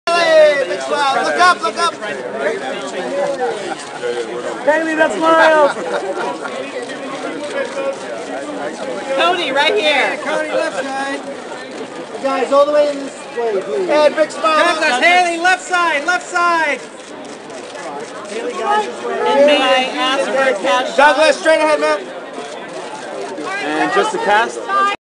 Ed Westwick at the 'Romeo & Juliet' Premiere in Los Angeles 3/5 - September 24, 2013
Ed Westwick (and the cast of the film) at the red carpet of the 'Romeo & Juliet' premiere celebrated in ArcLight Cinemas in Los Angeles, California on September 24, 2013. (Part 3).

This video belongs to: Getty Images Entertainment Video.

- Ed Westwick Spain - Web y fanclub en España del actor Ed Westwick.
¡VISÍTANOS!